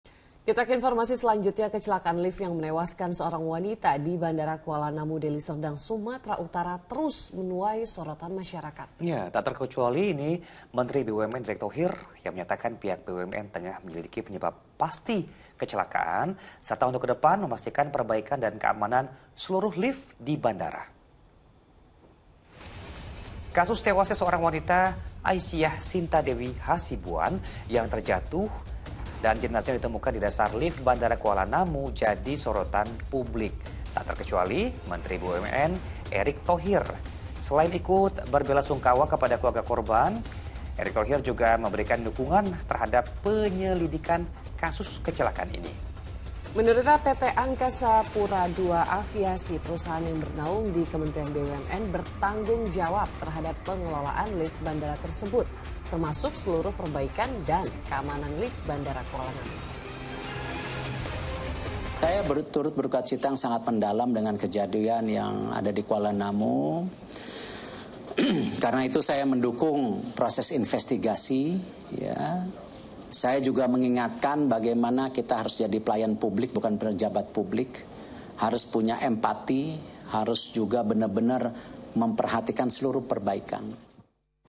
Menteri BUMN Erick Thohir Menyoroti Kasus Wanita yang Tewas Jatuh di Lift Bandara Kualanamu | Fokus
Kecelakaan lift yang menewaskan seorang wanita, di Bandara Kualanamu, Deli Serdang Sumatera Utara, terus menuai sorotan masyarakat. Tak terkecuali Menteri BUMN, Erick Thohir, yang menyatakan pihak BUMN tengah menyelidiki penyebab pasti kecelakaan, termasuk memastikan perbaikan dan keamanan seluruh lift di bandara tersebut.